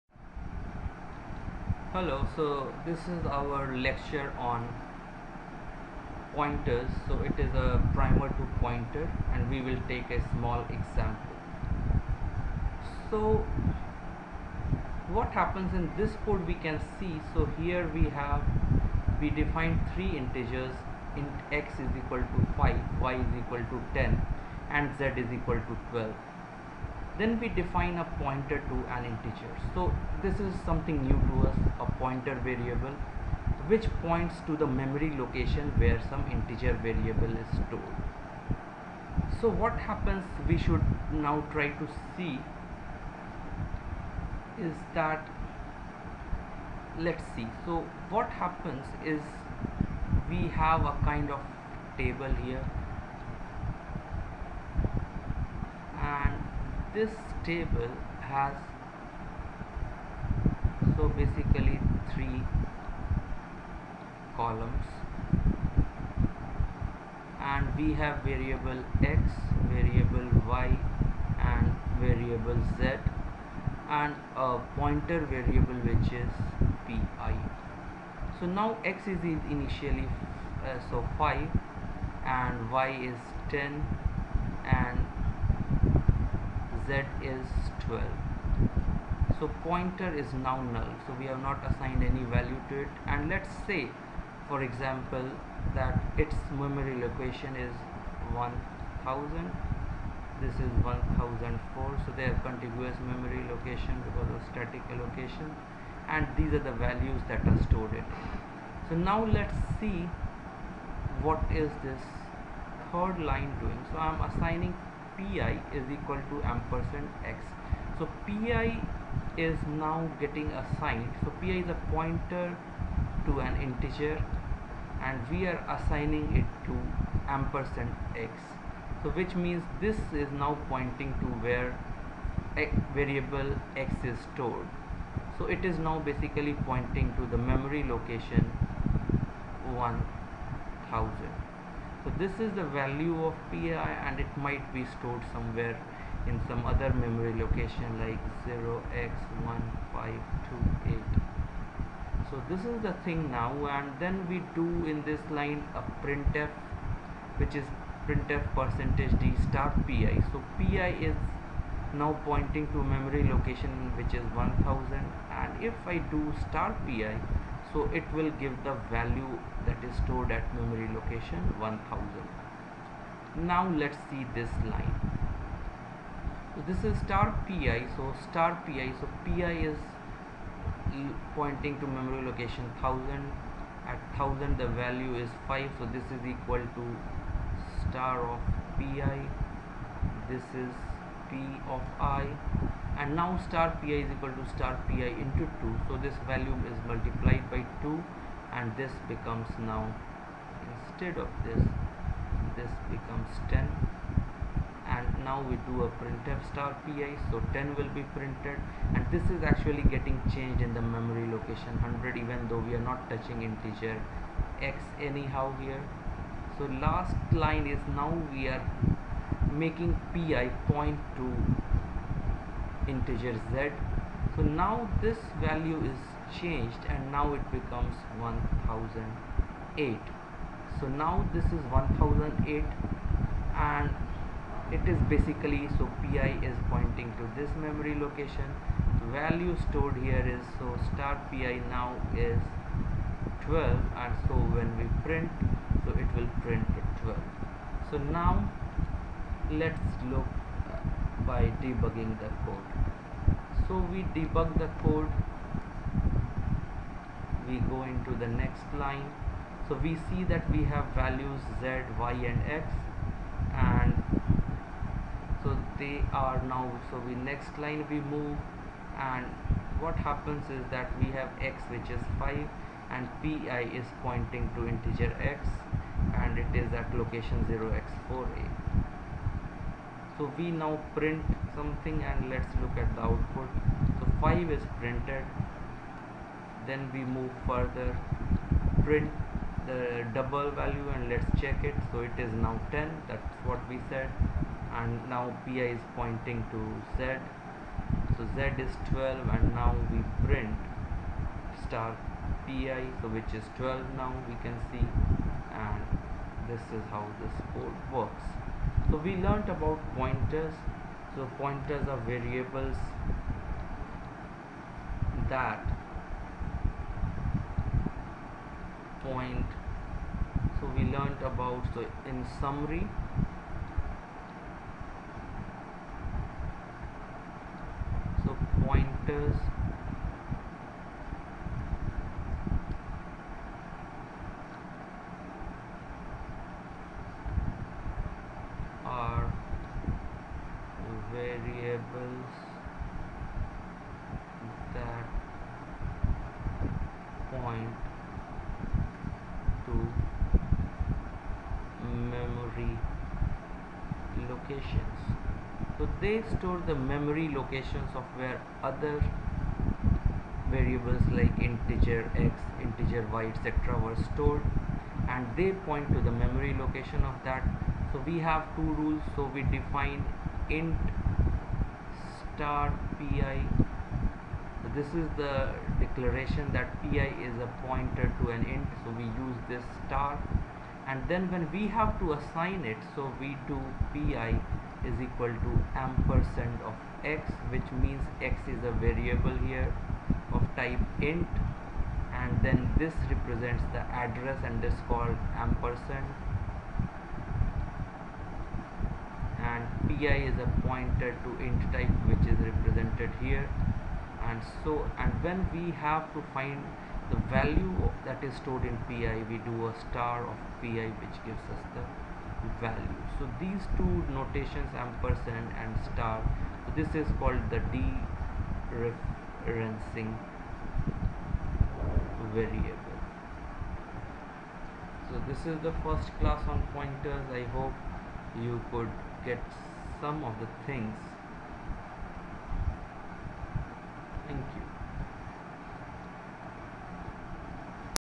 C Programming Tutorial: Pointer Example ( Referencing, De-referencing, ampersand, asterik))
This is the introductory lesson on pointers. It describes about pointers and how to use them. It shows in detail about memory map of pointers and variables along with the values stored in them